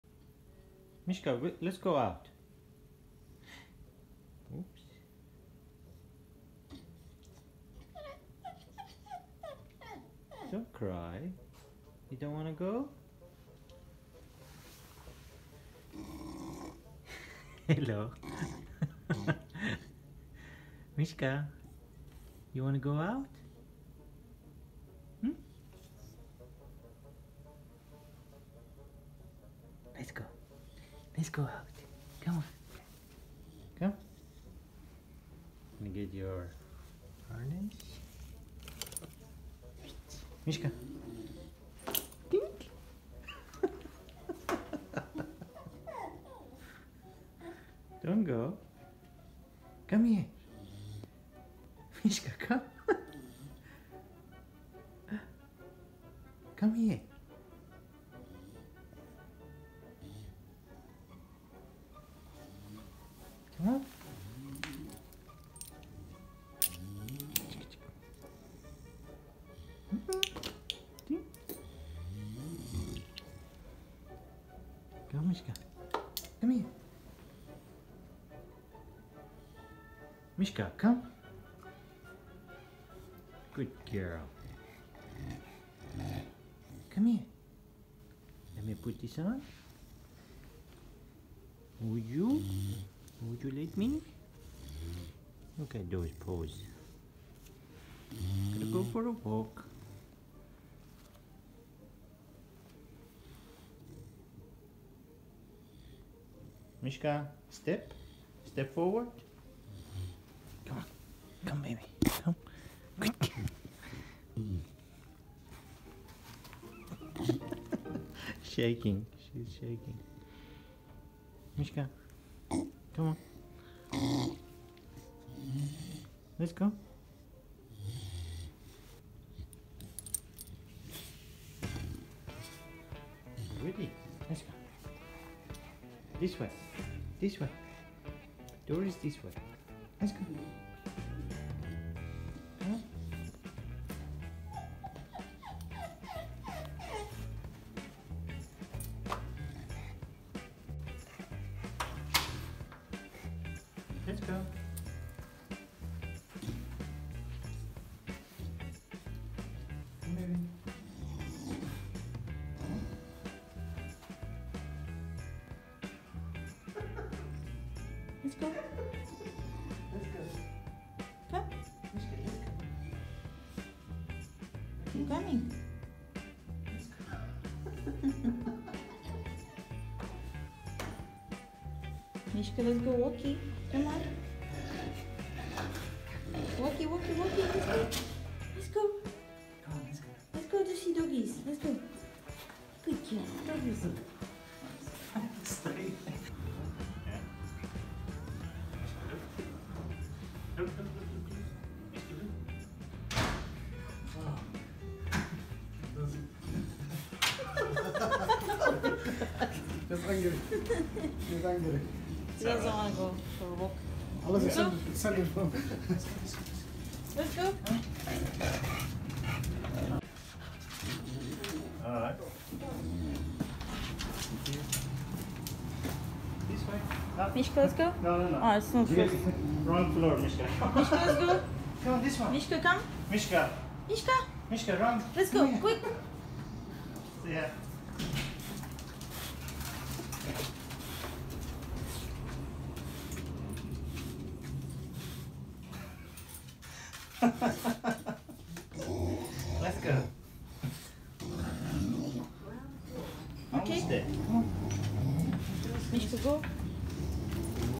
My Chow Chow doesn't want to go outside after the lockdown! DOG VLOG #3
Mishka doesn't like to go for a walk after Melbourne lockdowns. She's getting regular daily walks but it is not as easy as before.

Did she get lazy like her human staying at home?